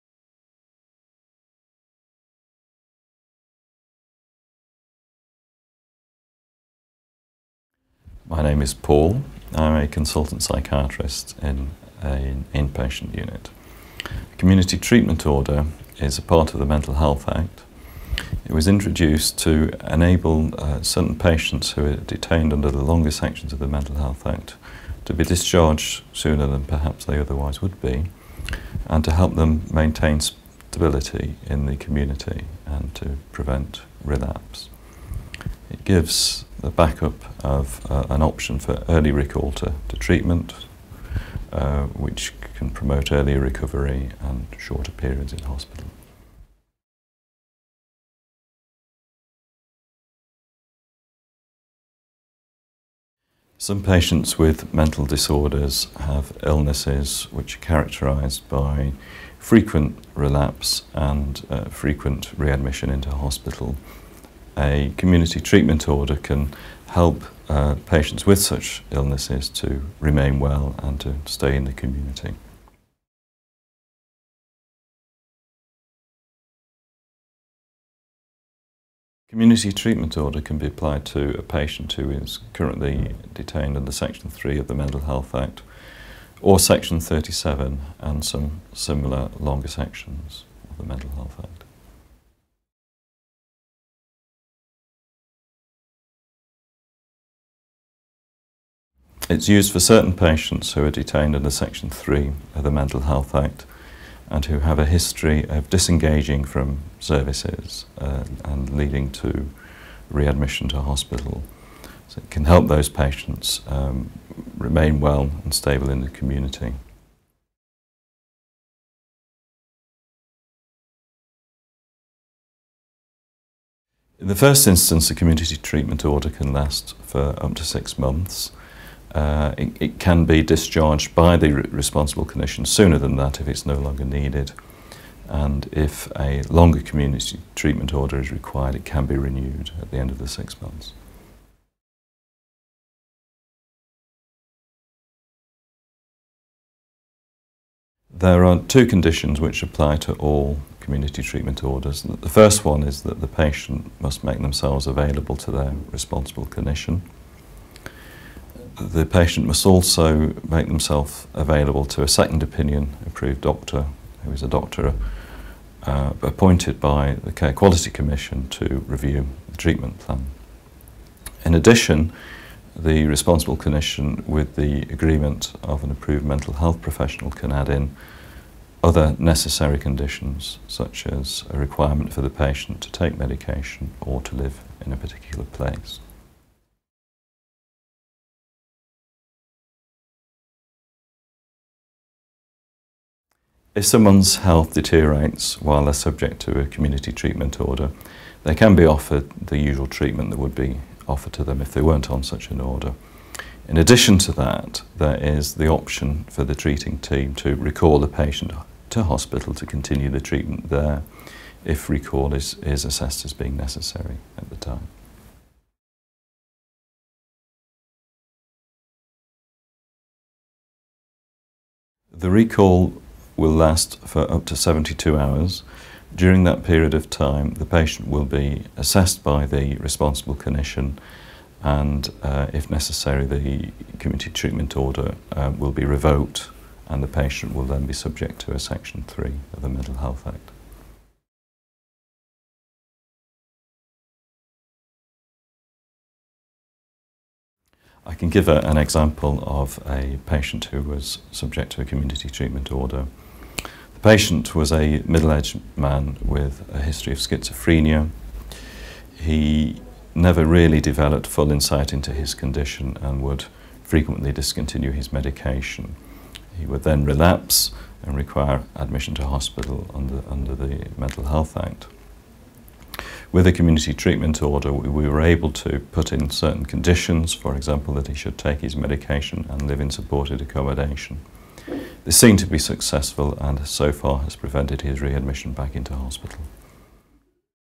Mental Health Act - What is a Community Treatment Order?
A Supervised Community Treatment Order is part of the Mental Health Act - it was introduced as part of the 2007 amendments and was designed to enable some service users who are detained under longer sections of the Mental Health Act to be discharged earlier if possible with conditions to support their stability and prevent relapse.  It gives a "back up" option of early recall to treatment which can promote early recovery and shorter periods in hospital.